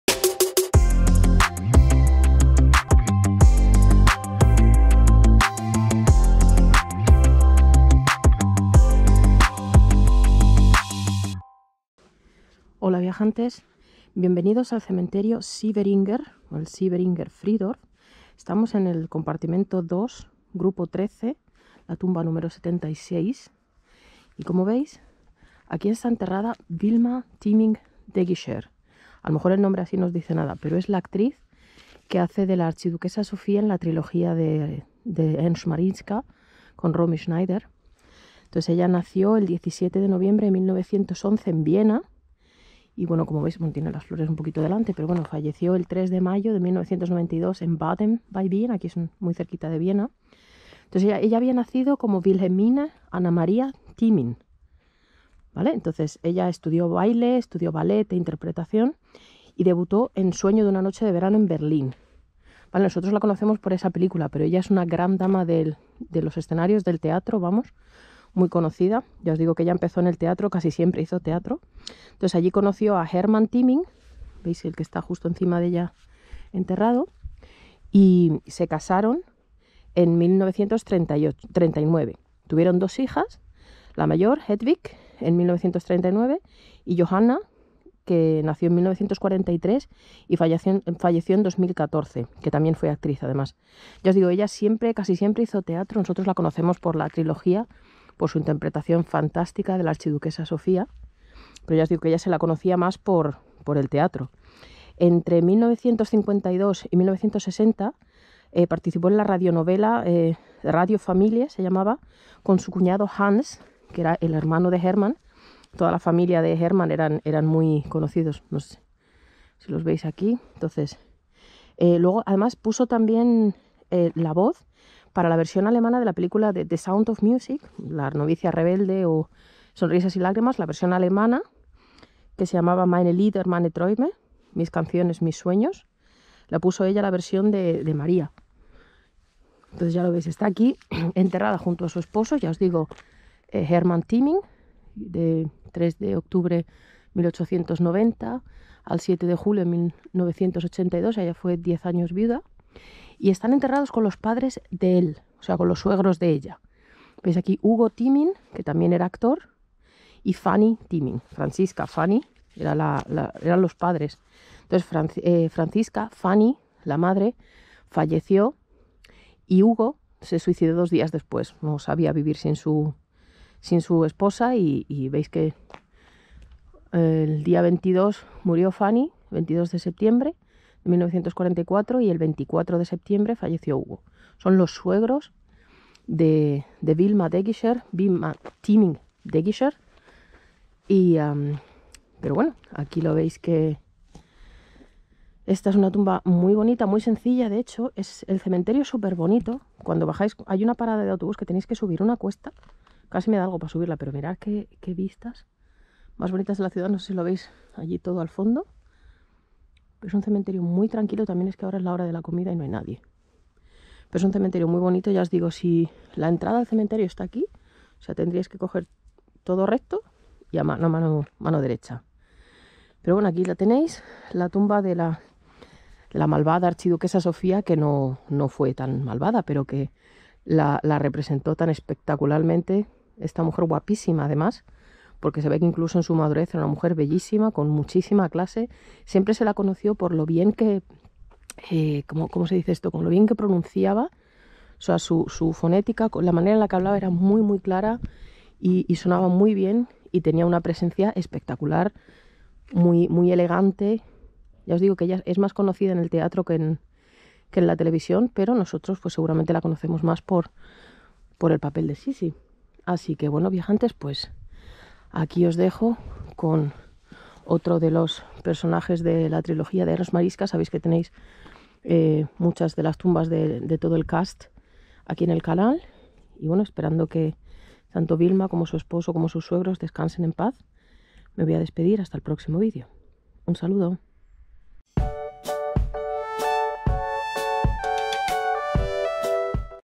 Tumba de VILMA DEGISCHER, La Archiduquesa Sofía de "SISSI"
Esta gran dama de la actuación fue la encargada de dar vida a la "malvada" Archiduquesa Sofía en la mundialmente conocida trilogía "Sissi".